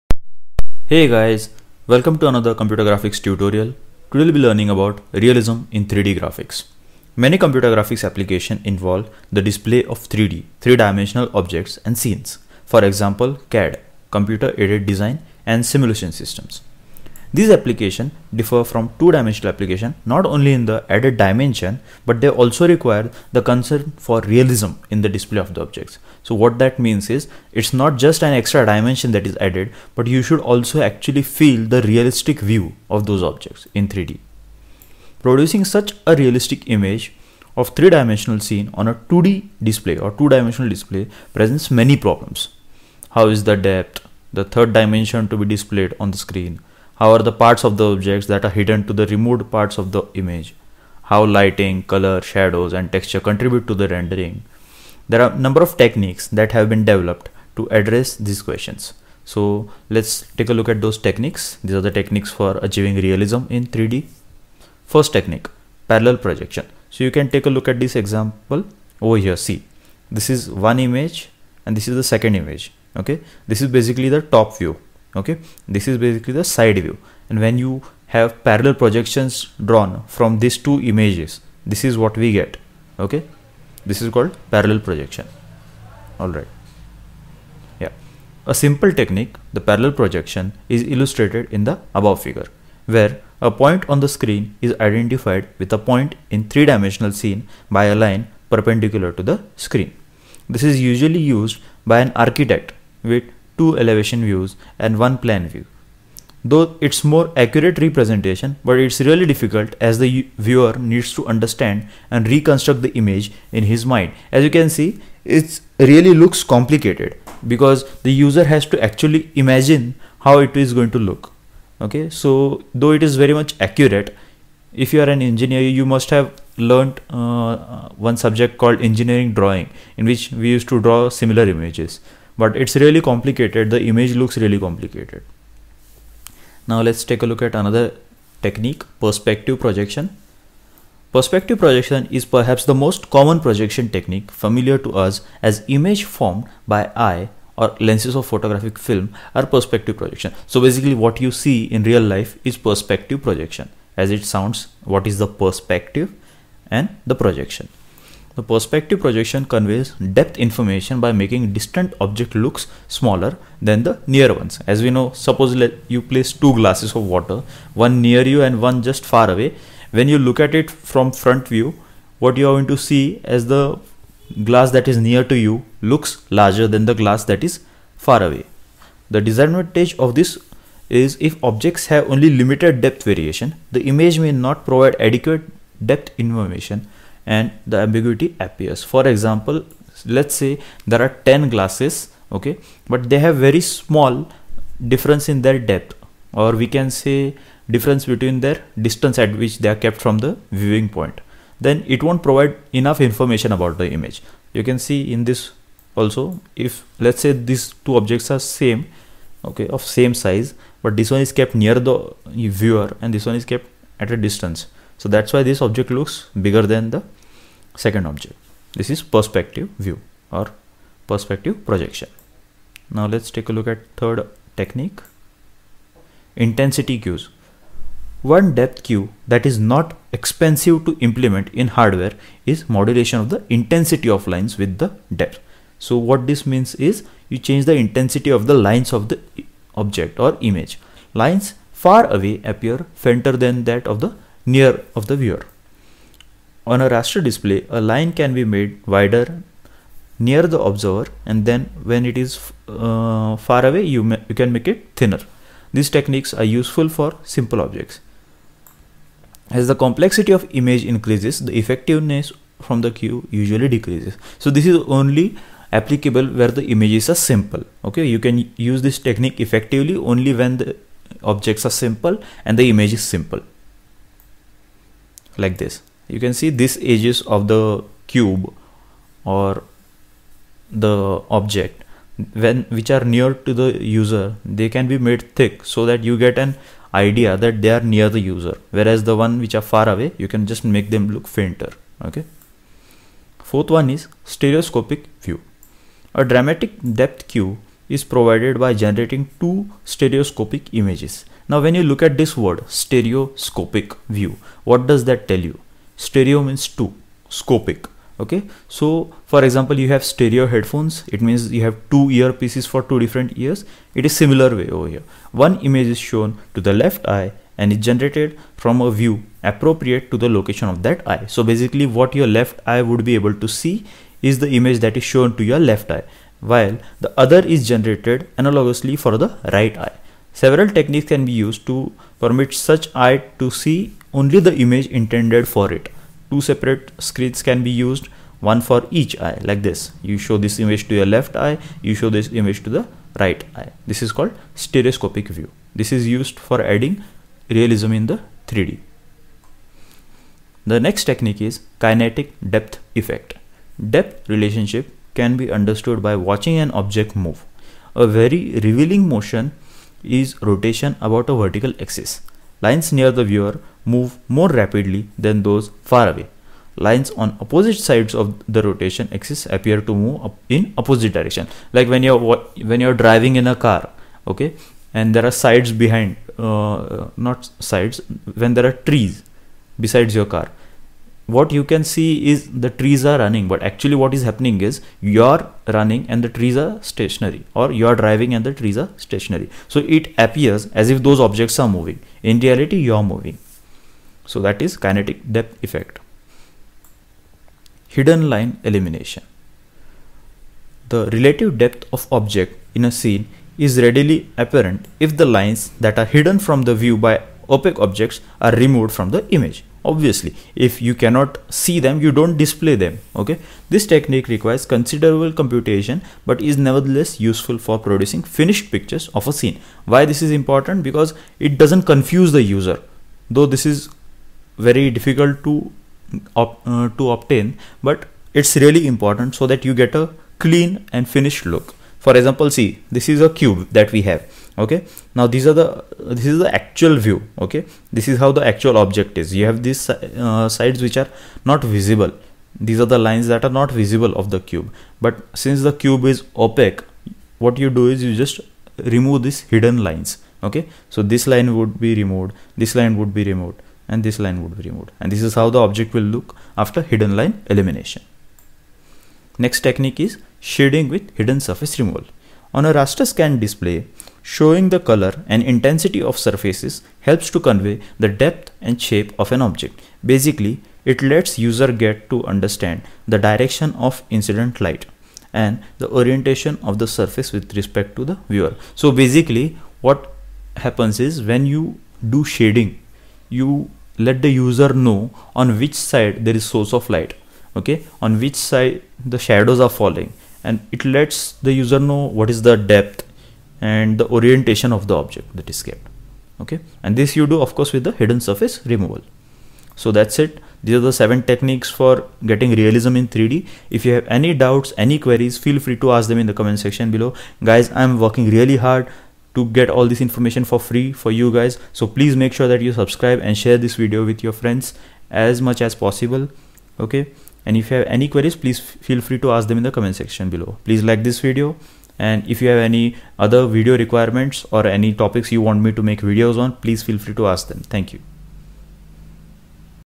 3D Projection in Computer Graphics | Parallel Projection & perspective projection
3D Projection in Computer Graphics: In this video we will learn about Parallel projection in computer graphics. We will learn about 3D projections in computer graphics like parallel projection, perspective projection, intensity queues. We will learn how to achieve realism in 3D computer graphics.
We will learn about hidden line elimination, kinetic depth in computer graphics, shading with hidden surface removal and stereoscopic views.